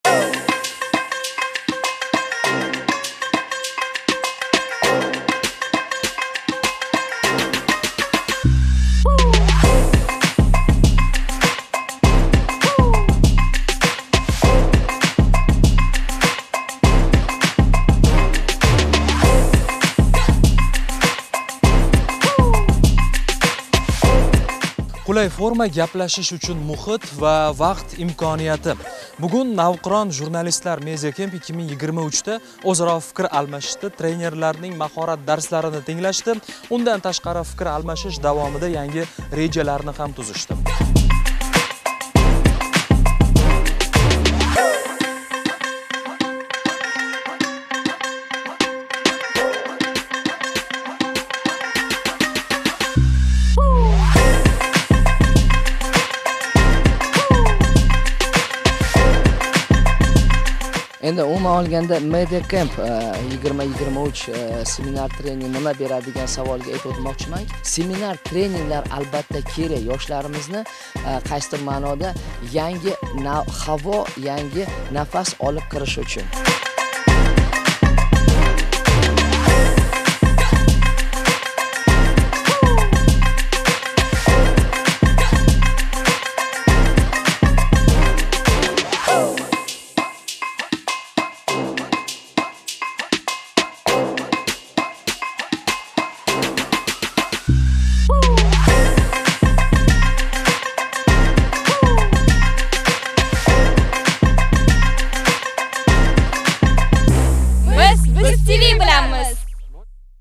Media camp qanday o’tdi?
"Media camp 2023" loyihasi doirasida Birlashmagan a'zo nodavlat teleradiokanallar ijodkorlari informatsion, tahliliy, ko'ngilochar dasturlar tayyorlash, yangi texnologiyalardan foydalanish bo'yicha Fransiya va Turkiyaning tajribali jurnalistlari, shuningdek Yunisef tashkilotining mutaxassislari hamda mahalliy soha vakillari orqali amaliy va nazariy bilimlarini boyitdilar. Shuningdek, jurnalistlar turli musiqiy-ko'ngilochar dasturlar, sport musobaqalari orqali madaniy xordiq olishdi. Media-oromgoh so'nggida jurnalistlar O'zMMB tomonidan sertifikat va esdalik sovg'alar bilan taqdirlandi. Media oromgoh Toshkent viloyati Bo'stonliq tumanida joylashgan Ugam oromgohida tashkil etilib, unda 100 dan ortiq ijodiy va texnik xodimlar ishtirok etdi.